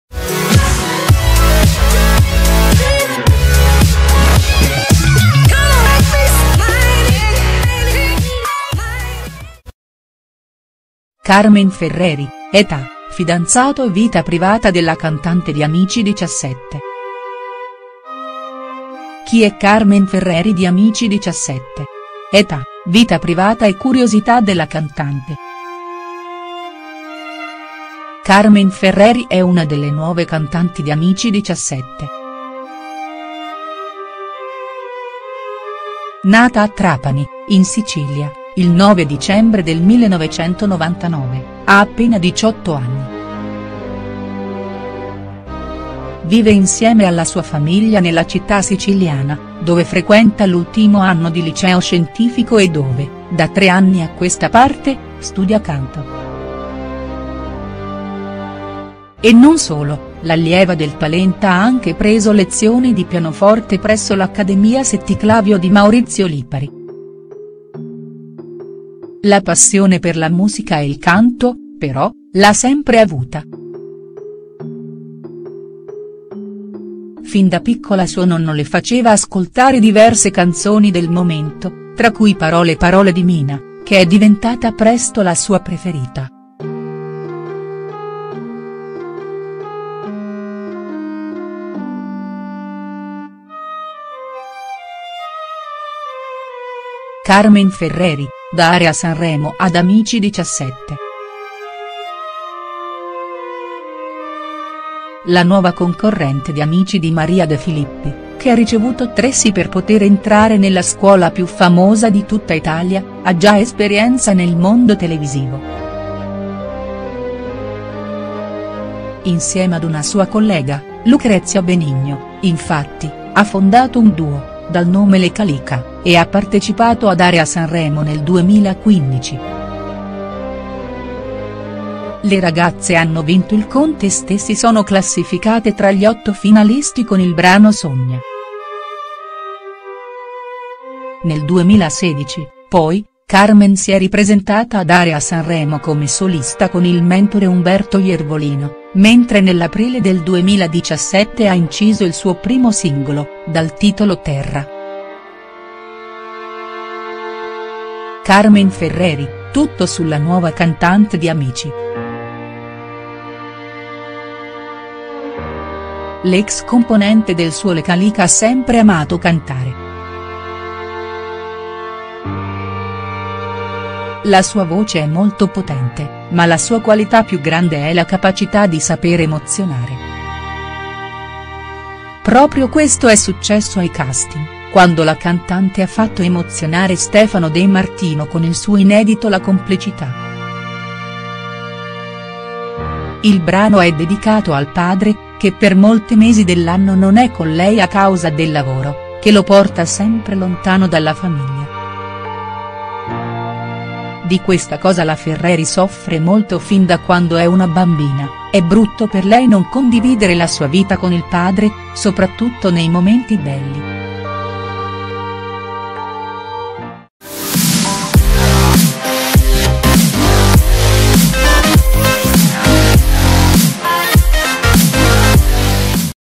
Carmen Ferreri: età, fidanzato e vita privata della cantante di Amici 17| STARS NEWS
Leggi su Gossipetv tutto su Carmen Ferreri: età, vita privata, carriera e altre curiosità della cantante del talent Amici 17